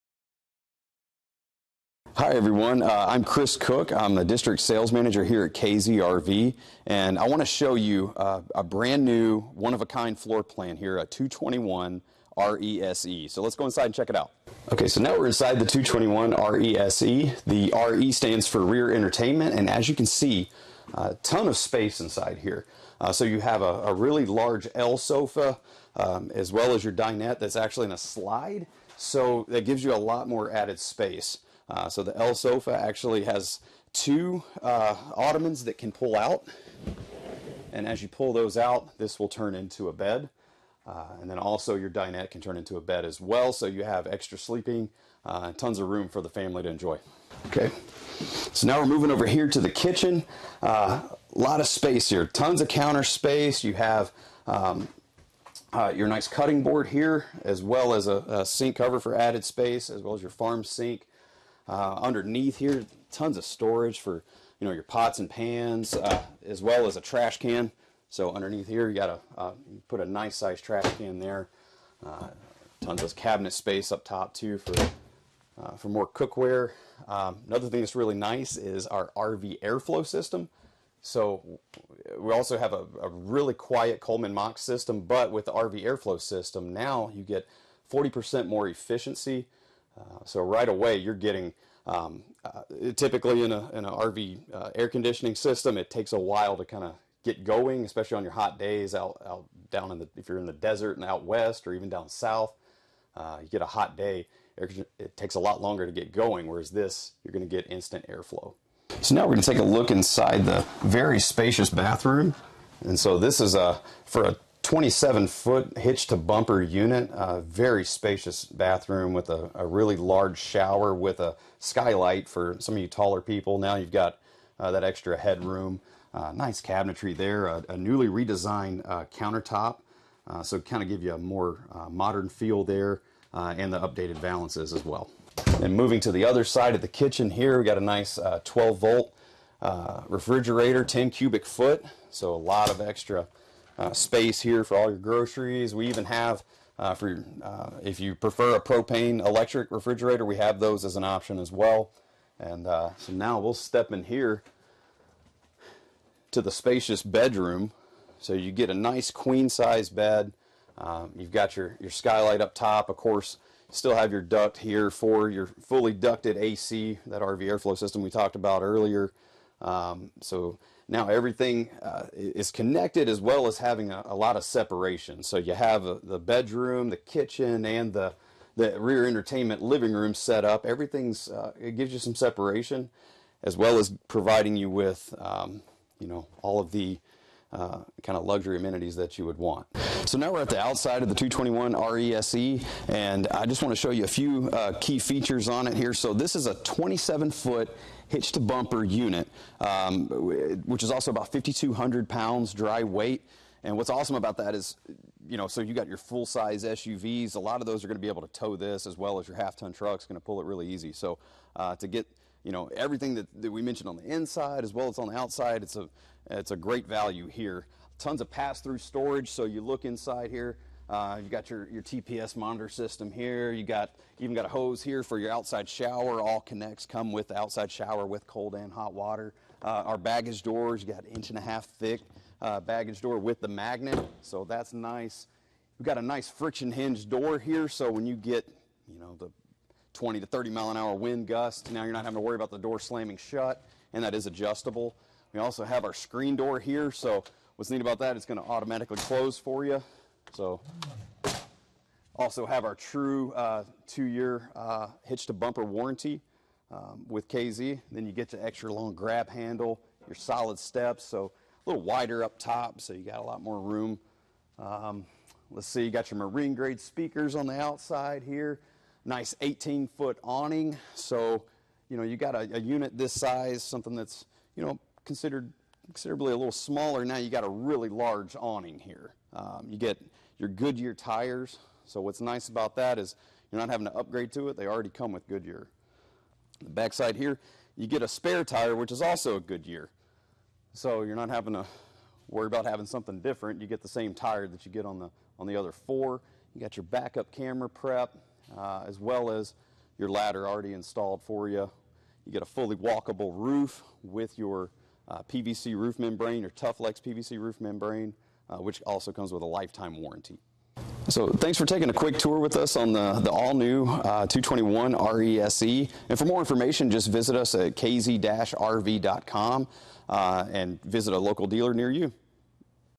2023 KZ RV Connect® SE C221RESE Travel Trailer Guided Tour
View the exterior and interior features of the 2023 Connect SE C221RESE lightweight travel trailer from KZ RV. For more information on Connect SE lightweight travel trailers visit: